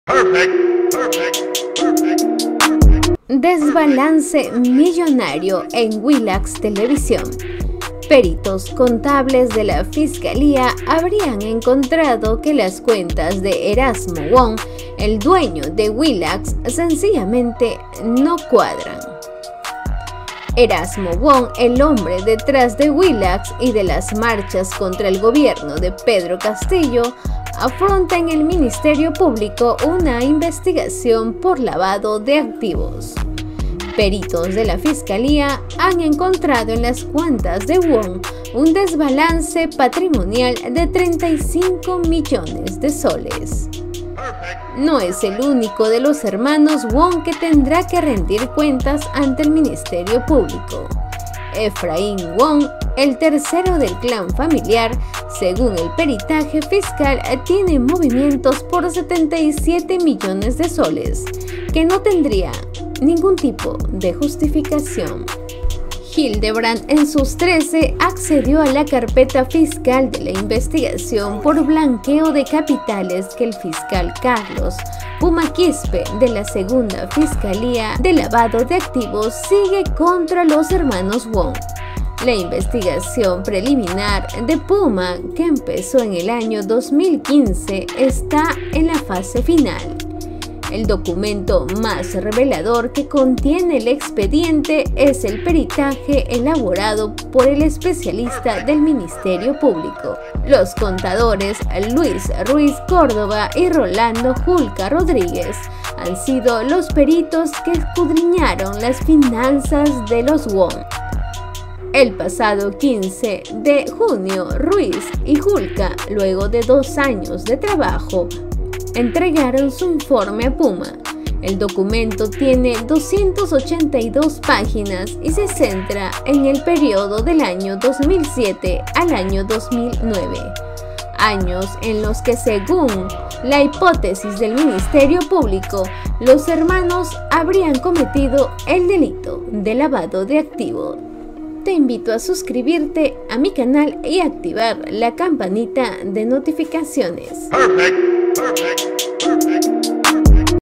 ¡HUNDEN A WILLAX!CUENTAS DE ERASMO WONG NO CUADRAN Y HABRIA DESBALANCE DE s/.35 MILLONES
Hildebrandt en sus trece saca a la luz la investigacion al caso del desbalance patrimonial de S/. 35 millones en los hermanos wong, dueños de Willax tv.